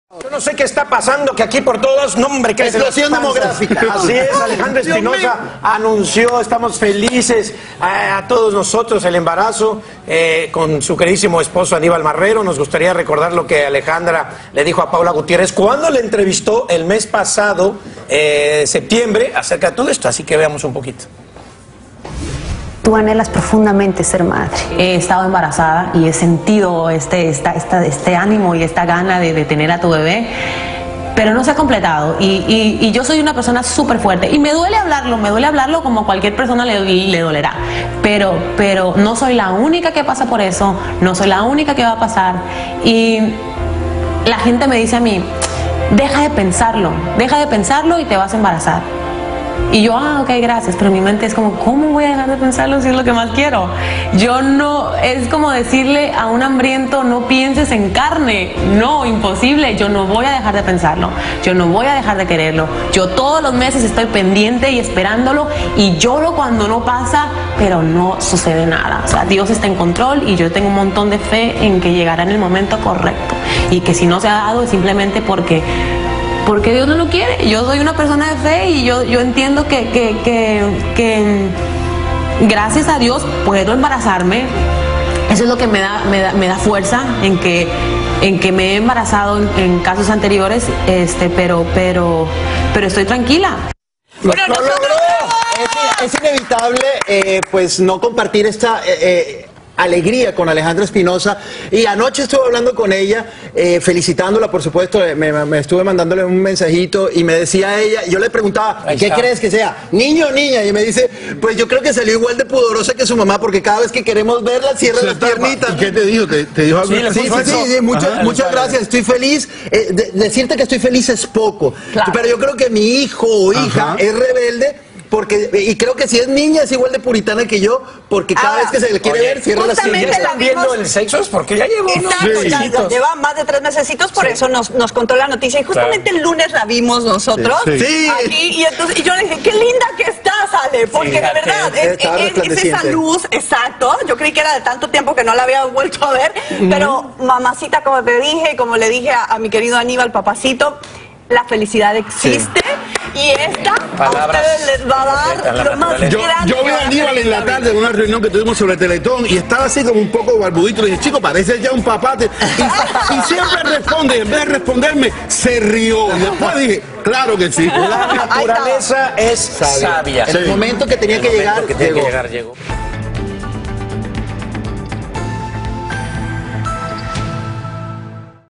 Alejandra Espinoza:"Tengo fe en que el tiempo de Dios es perfecto"
Alejandra Espinoza habló con Paola Espinoza sobre su deseo de ser madre y cómo le dolía no quedar embarazada. Ahora celebra su embarazo feliz.